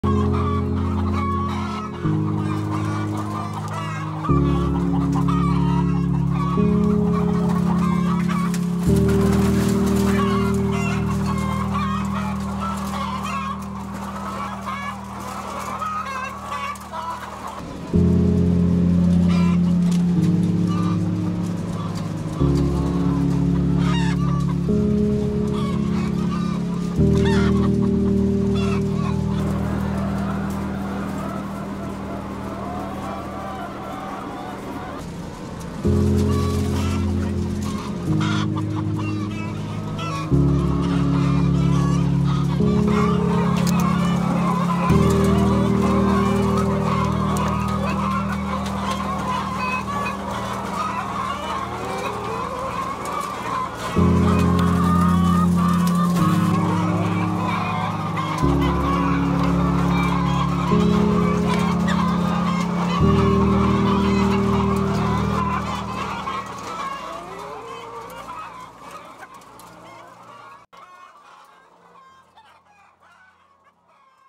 Œufs de poules en cage: Marie Blachère, engagez-vous!
Les boulangeries Marie Blachère n'ont aucun engagement public à cesser d'utiliser des œufs de batterie pour la préparation de leurs produits, contrairement à Paul, Brioche Dorée ou La Croissanterie. Il est temps que cette enseigne évolue!